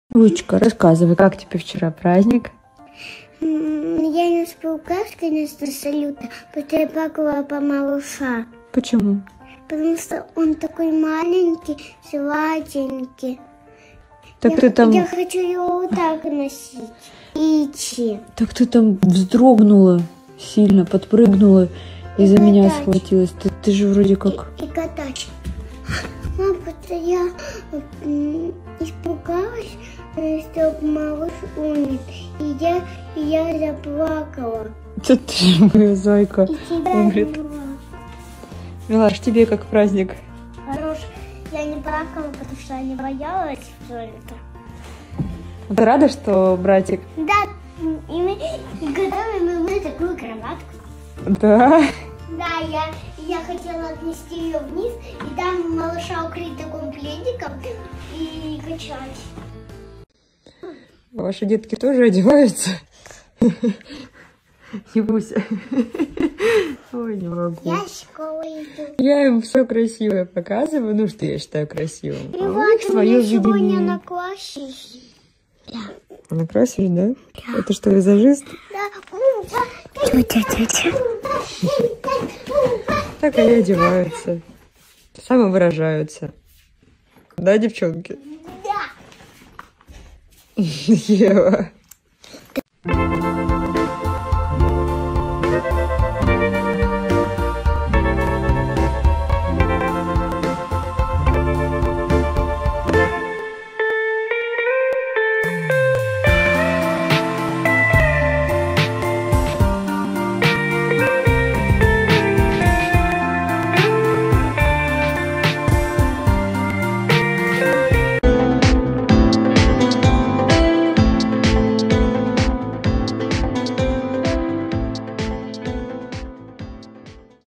Дети Тарасова и Костенко рассказали, что думают о рождении нового ребенка и о испуге от гендер-пати
Анастасия Костенко и Дмитрий Тарасов в минувшую пятницу устроили гендер-пати, на которой впервые объявили пол будущего ребенка (четвертого по счету). Вечеринку супруги устроили скромную, только для близких, однако все равно не поскупились на украшения, установленные во дворе их загородного дома.